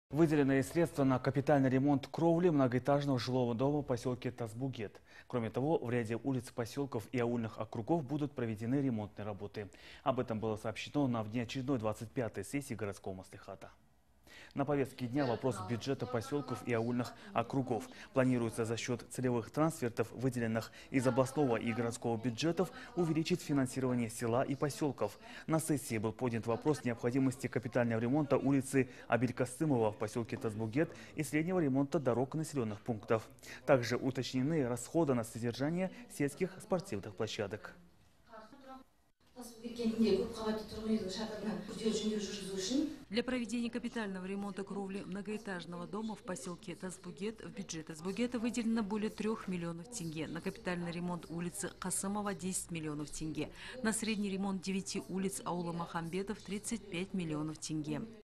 СРЕДСТВА НАПРАВЛЕНЫ НА РЕШЕНИЕ АКТУАЛЬНЫХ ПРОБЛЕМ
Выделены средства на капитальный ремонт кровли многоэтажного жилого дома в поселке Тасбогет. Кроме того в ряде улиц поселков и аульных округов будут проведены ремонтные работы. Об этом было сообщено на внеочередной 25 сессии городского маслихата.